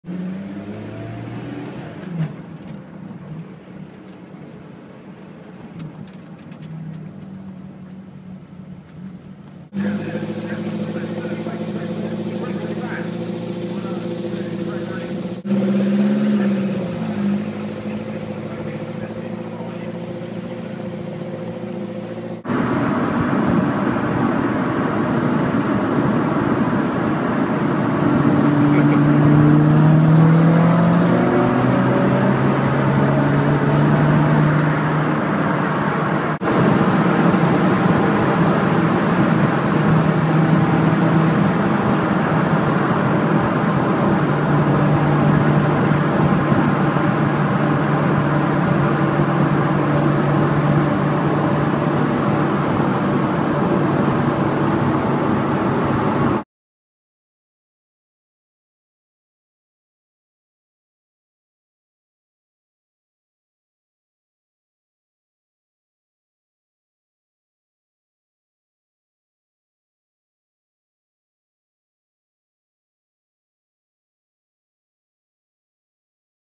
Lotus Esprit Sport 300 and Exige V6 Cup in formation.
There aren't nearly enough Sport 300 videos on youtube. I know they are super rare but really, come on youtube, sort it out!

This very mean, very black example is car number 63 of 64 built. It had just under 31500 miles on the clock at the time of shooting. Here it is leaving the Brands Hatch Festival Sunday 25th August 2013. Very much Lotus's F40, it still has incredible presence on the road. Easily the greatest 4cyl supercar of all time and one of the greatest Lotus cars ever.

Shot from inside an Exige V6 Cup so this is a formation of ultra rare Lotus goodness!